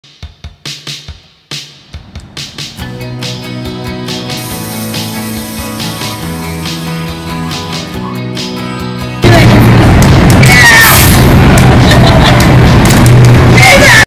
GISH2018 Team MOTHRA - Item 4
We can’t stand idly by while commuters deal with stress & boredom! Set up a “Bubble-wrap Stress Relief” station at a subway or bus station.